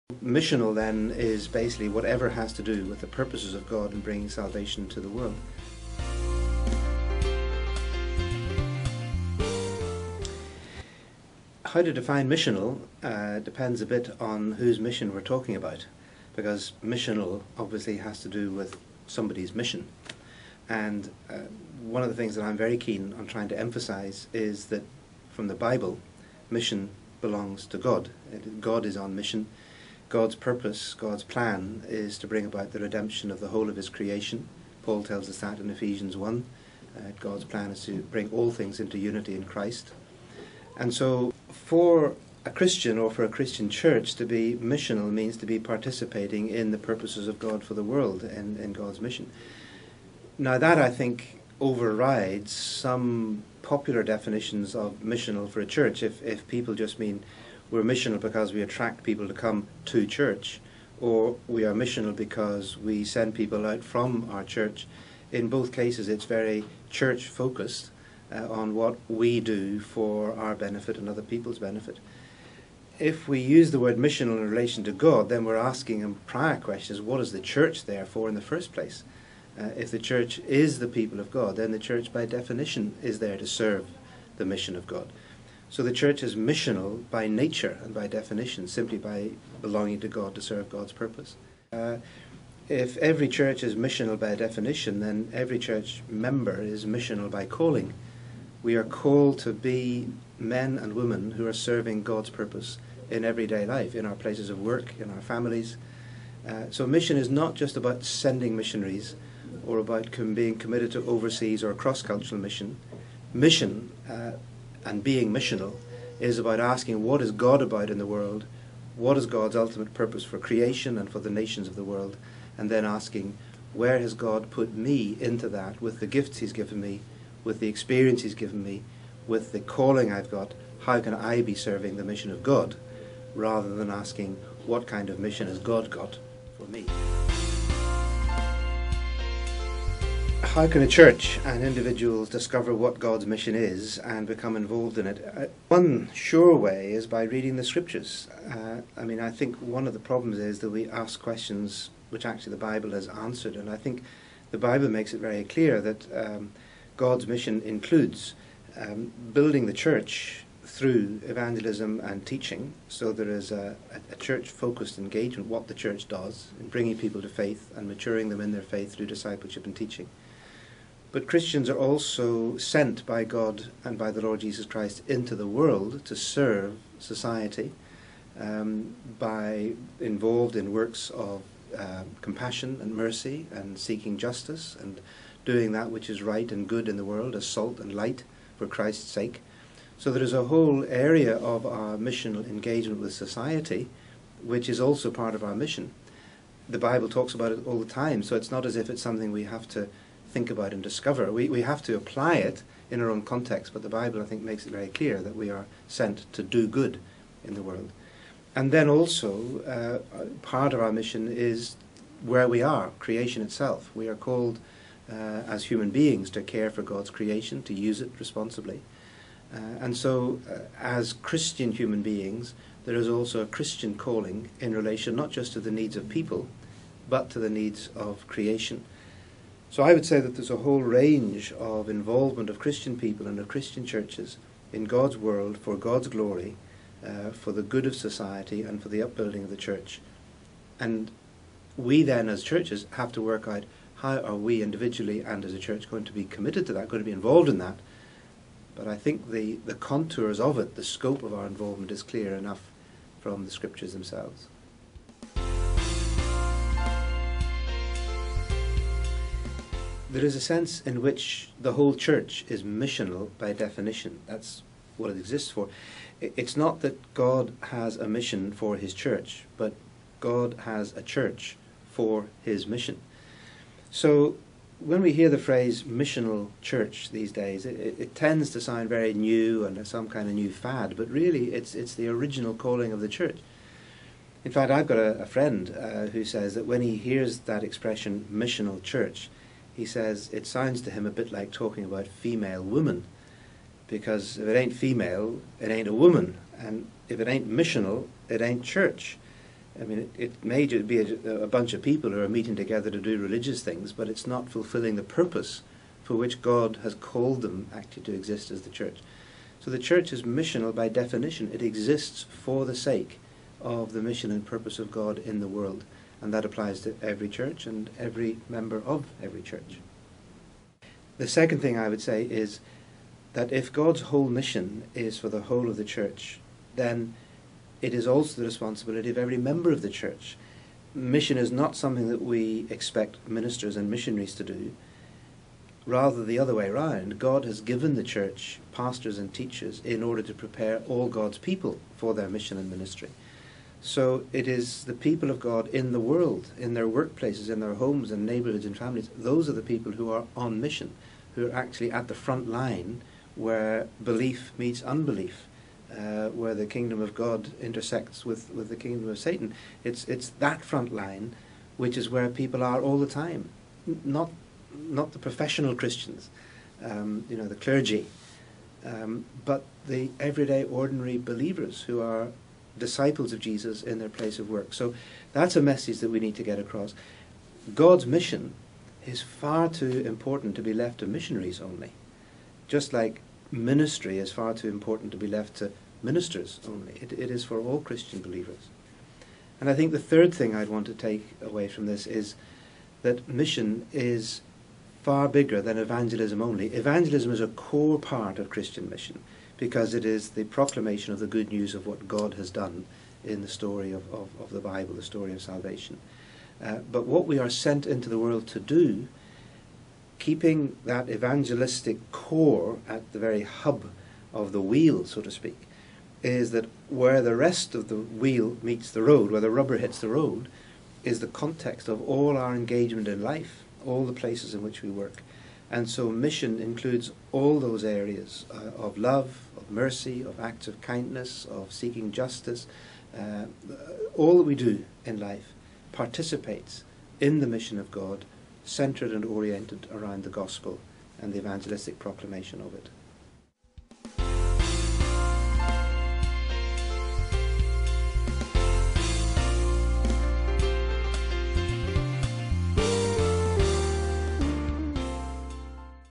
Interview with Christopher J. H. Wright
An interview with Christopher J. H. Wright about missional churches at the Evangelical Presbyterian Church's 32nd General Assembly in Baton Rouge, LA at First Presbyterian Church (June 19th, 2012)